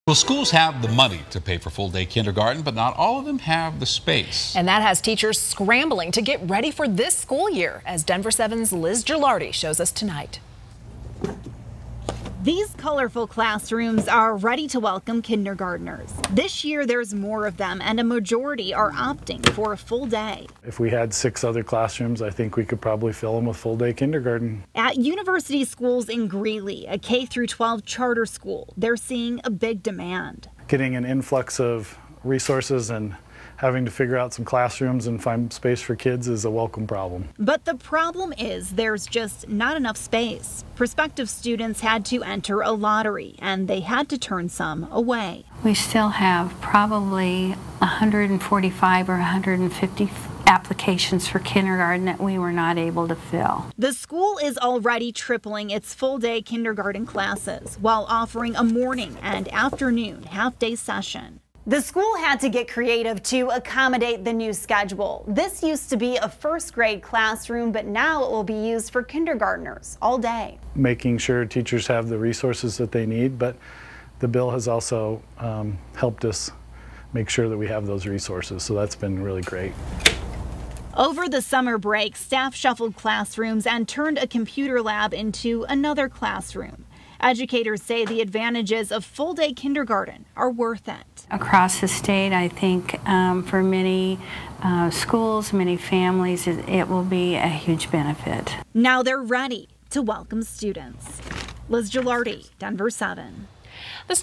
Educators welcome full-day kindergarten but struggle to find classroom space at Greeley school
As students head back to school, there's a challenge to find room for kids in full-day kindergarten.